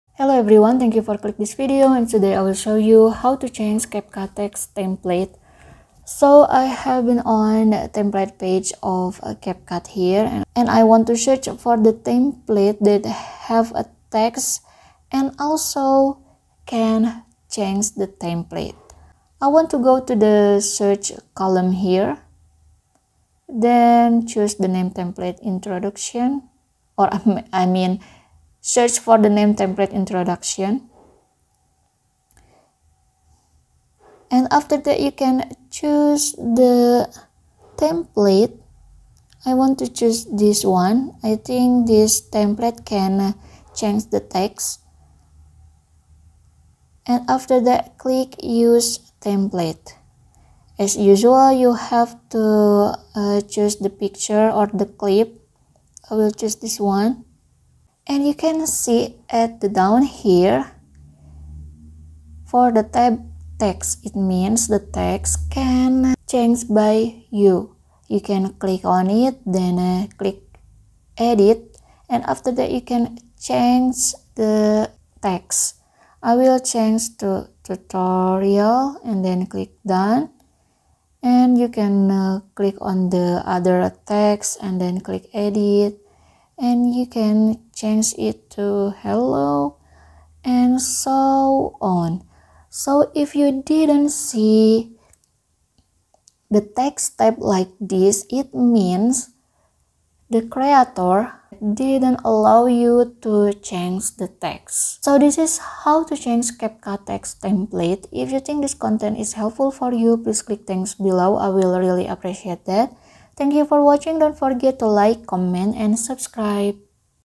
How To Change Text Template In CapCut
Hello everyone!

Thank you for click this video. Today I will show you how to change text template in CapCut.